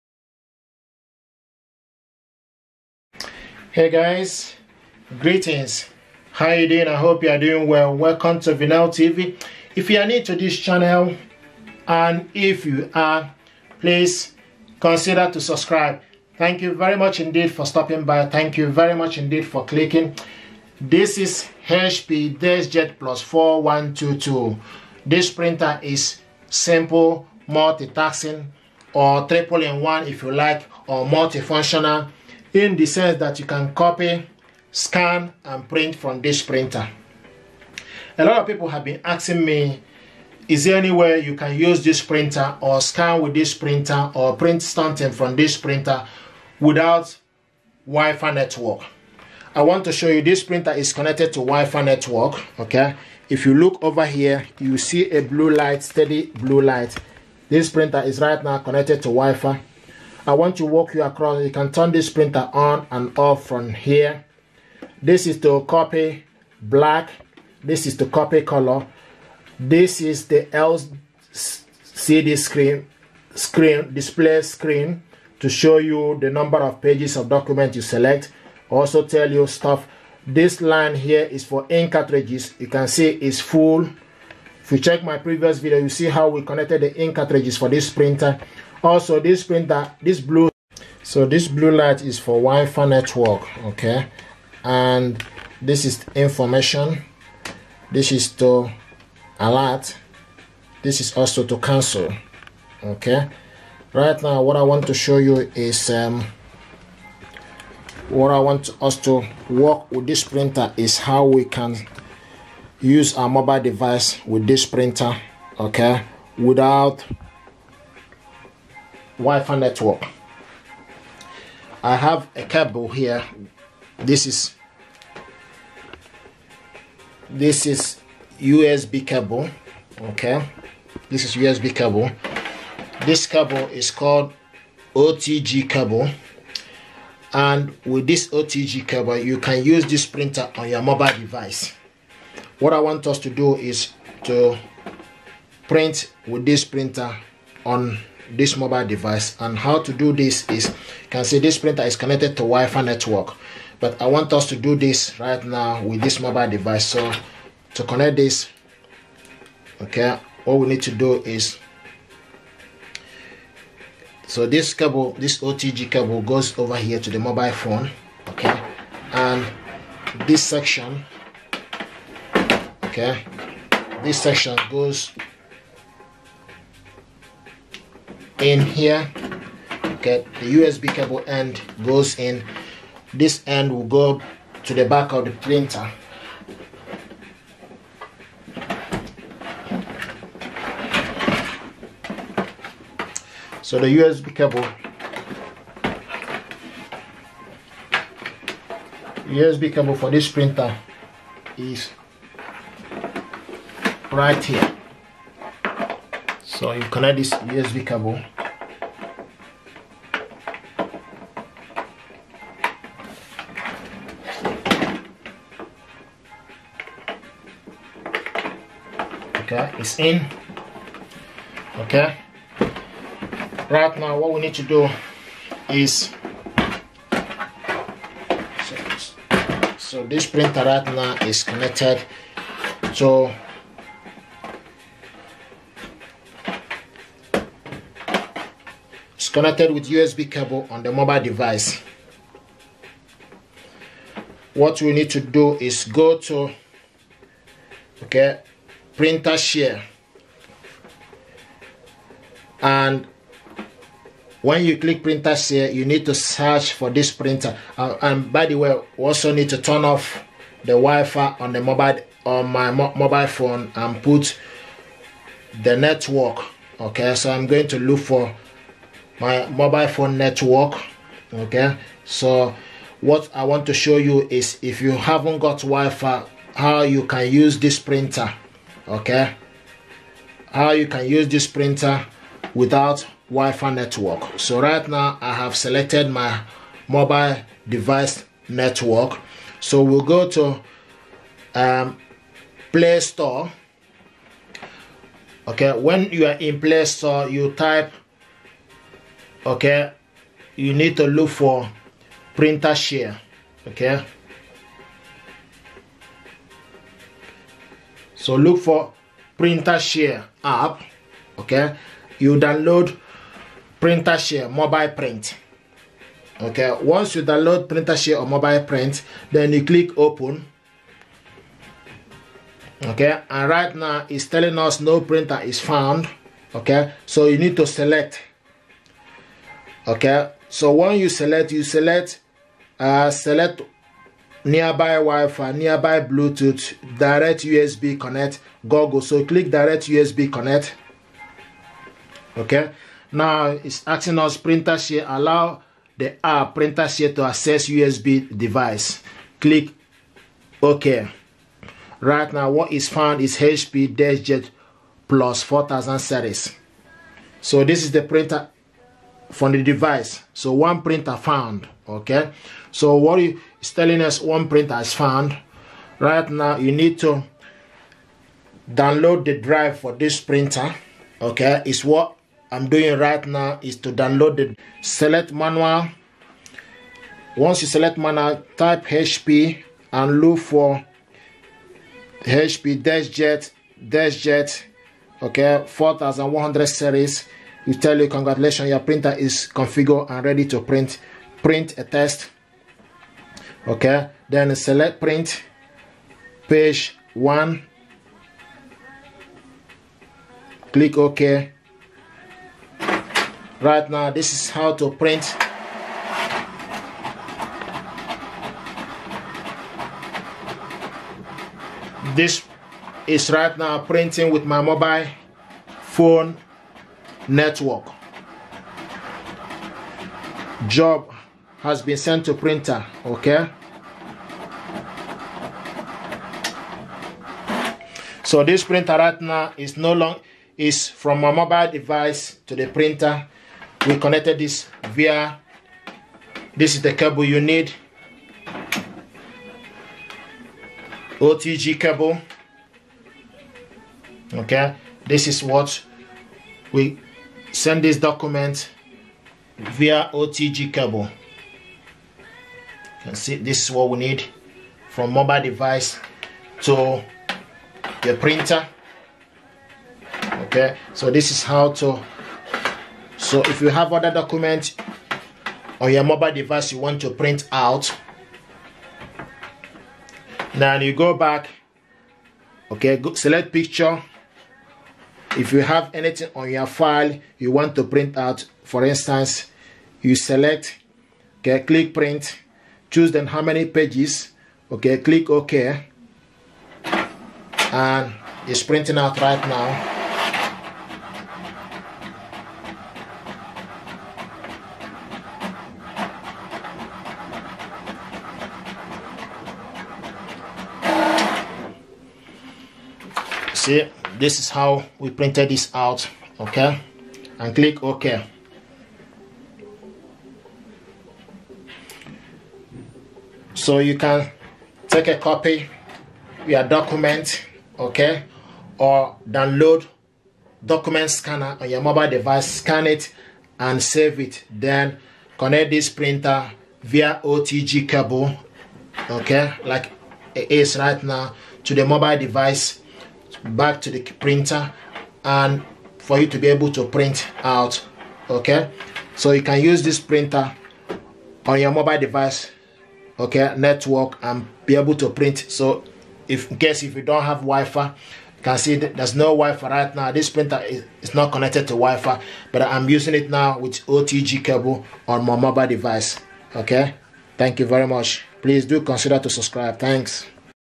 HOW TO CONNECT HP DESKJET PLUS 4122 TO MOBILE DEVICE USING ON-THE-GO (OTG) CABLE NO WIFI NEEDED
THIS VIDEO WOULD SHOW YOU HOW TO CONNECT HP DESKJET PLUS 4122 WITH YOUR MOBILE PHONE DATA TO PRINT WITHOUT THE NEED OF WIFI CONNECTION. YOU CAN PRINT AND SCAN WITH THIS INKJET PRINTER.  IT COMES WITH BUILT-IN WIFI, SO YOU CAN USE IT WITHOUT NEEDING TO PLUG IT INTO YOUR COMPUTER AND WHETHER  IT'S BUSINESS REPORTS  OR CAKE-SALE FLYERS, YOU'LL GET SHARP TEXT, BOLD BLACKS AND CRISP COLOUR  IMAGES  WITH EVERY PRINT. SO THIS DESERVE YOUR  ATTTENTION. THIS VIDEO ALSO WORKS FOR FOLLOWING PRINTERS :